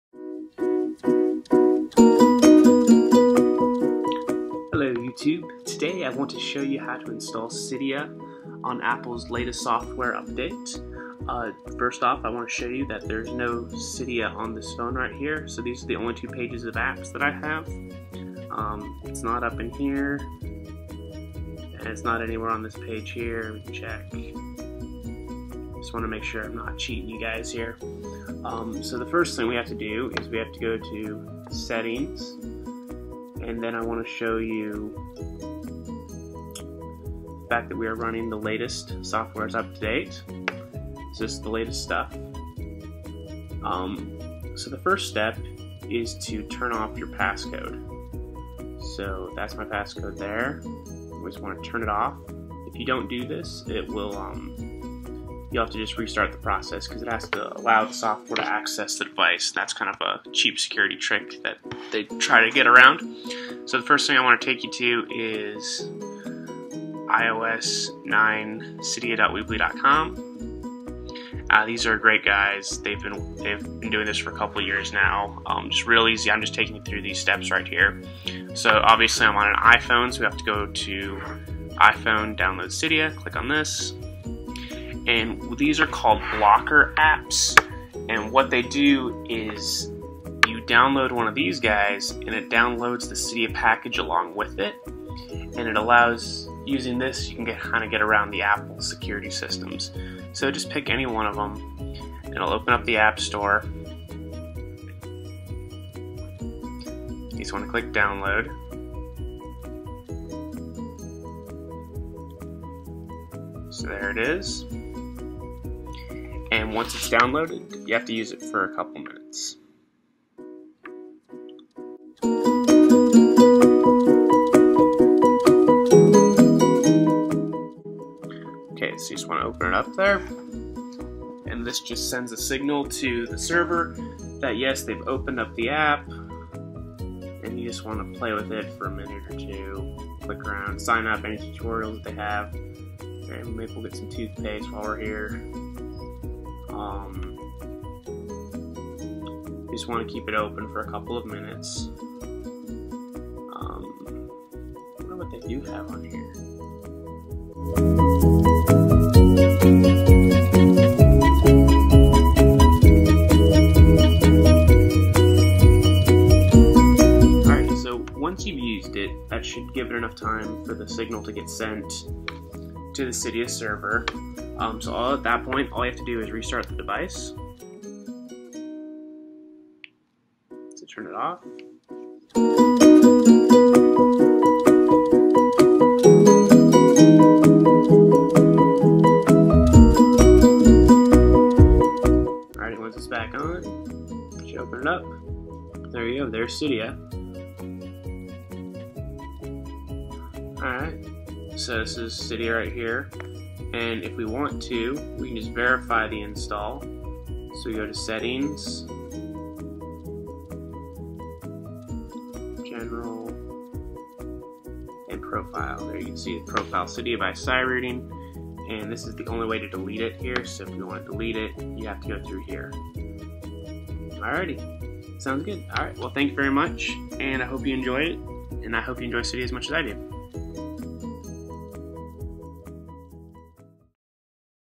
[OUT NOW] Iphone Cydia Install on IOS 9.3.2/9.3.3 Jailbreak Release For iOS 9.3.2/9.3.3
New iOS 9.3.2 & 9.3.3 Cydia install Released! Working on all iPhones, iPads, and iPods.
1. Download one of the applications below.
2. Open application and run for at-least for 5 minute.
3. Close application.
4. Restart your iOS device.
5. Cydia will now be on your home screen! Enjoy!